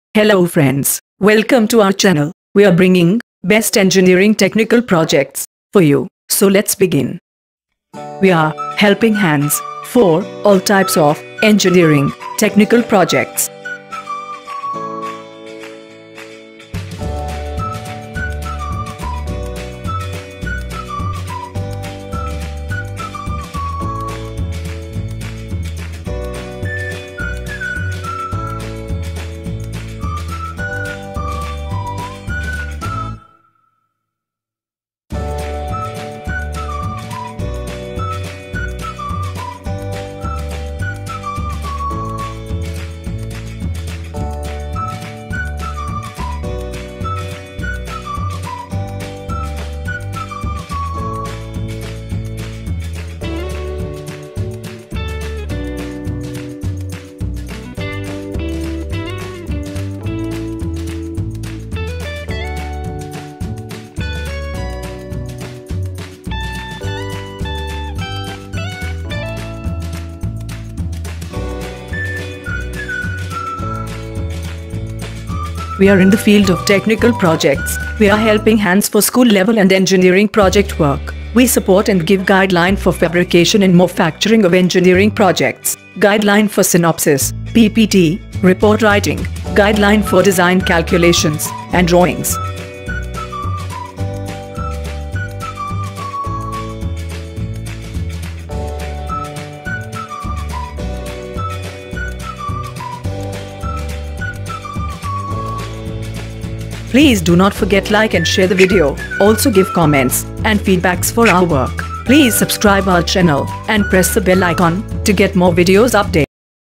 PNEUMATIC ROBOTIC ARM | ROBOT ARM | SIMPLE ROBOT ARM| MECHANICAL ENGINEERING PROJECT
Hello Friends,
In this video, we will see, the project on robotic arm, for different industrial applications. majorly this developed model, we can use in many industry, for different application. In present conditions, many automatic and hydraulic machinery are available, but they are not affordable for small foundries,  So by observing, these problems, we have tried to develop a simple model for pick and place application. The development cost of this machine is very low,  the machine can be handled by unskilled labour and it is very easy to use. The video is showing the working and testing of the machine.